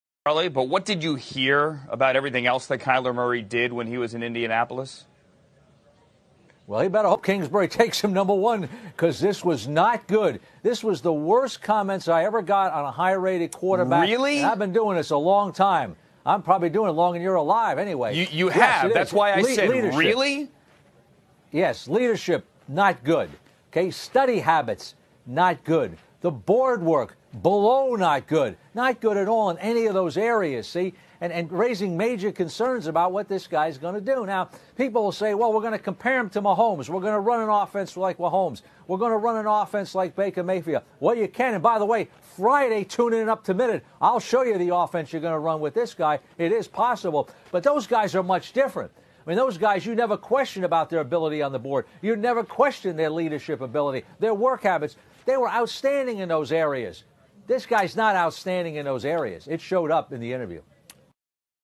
Charley Casserly: Comments from teams on Kyler Murray were worst he has heard
Reports from several NFL teams that spoke to Kyler Murray at the Combine revealed that in their opinion Murray has some major flaws